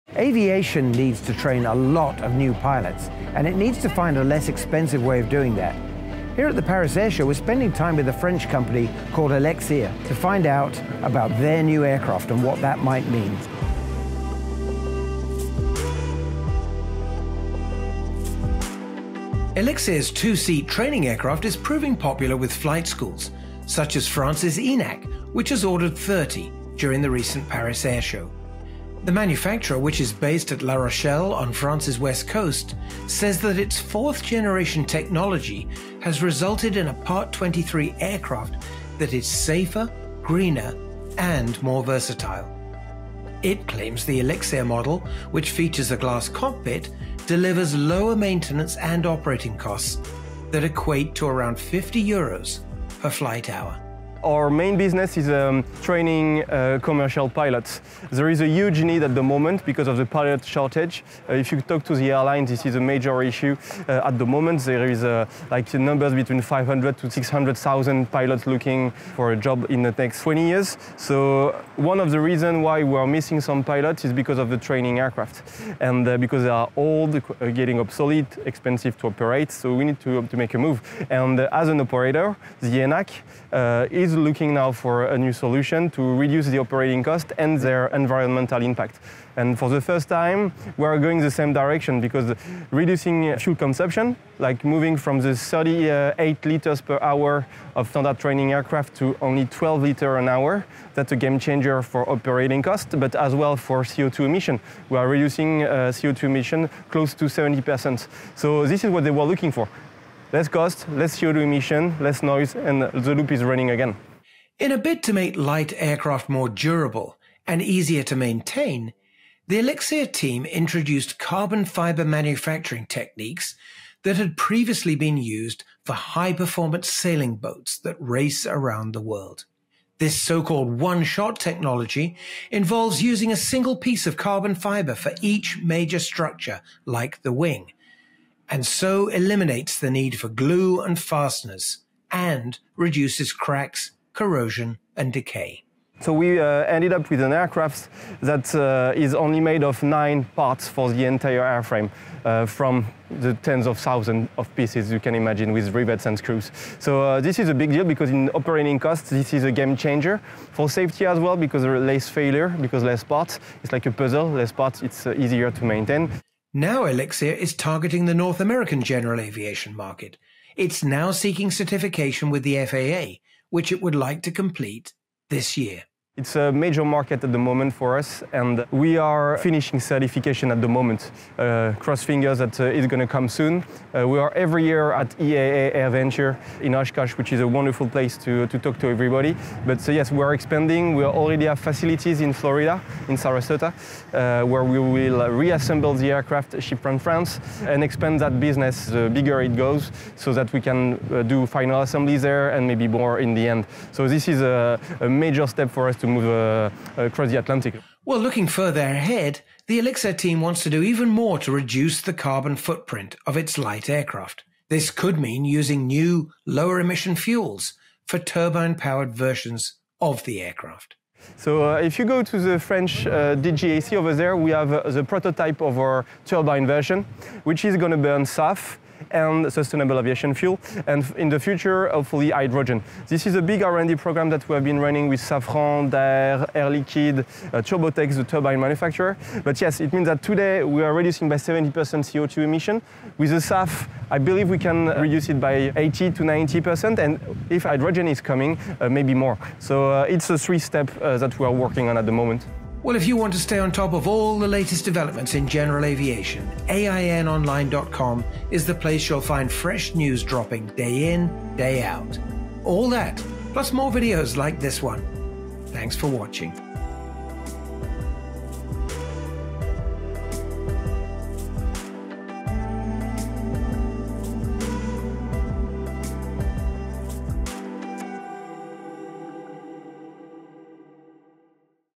Elixir Aircraft Offers a New Approach to Flight Training with High Tech Carbon Fiber Airframe – AIN
Founded in 2015, Elixir Aircraft produces the 100-hp Elixir, a fourth-generation aircraft certified in 2020. Built using a OneShot carbon composite structure, the aircraft consists of nine primary parts, eliminating the need for rivets or glue. This streamlined design increases safety, simplifies maintenance, and reduces fuel consumption. Combined with an ultra-efficient engine, the Elixir offers up to 70% fewer CO2 emissions compared to conventional trainers.

Elixir Aircraft secured an order from the École Nationale de l'Aviation Civile (ENAC), France’s national civil aviation school, for 30 two-seat trainers at the recent Paris Air Show. The order, part of ENAC’s fleet renewal initiative, will see deliveries between 2026 and 2027 and marks a key milestone for Elixir’s expansion in institutional aviation markets.

Co-founder Cyril Champenois told AIN that the company’s journey began when he and his friends took a design they initially used for boat racing and applied it to aviation. “They needed those to do races around the world at 30 knots without breaking anything,” he said. “Now we are working in aviation trying to make sure that we are not breaking anything.”

for all the latest on the aviation industry, and *AIN* sister for the most comprehensive resource available for anyone using—or interested in trying—private flight. for all the news on developing aviation technology.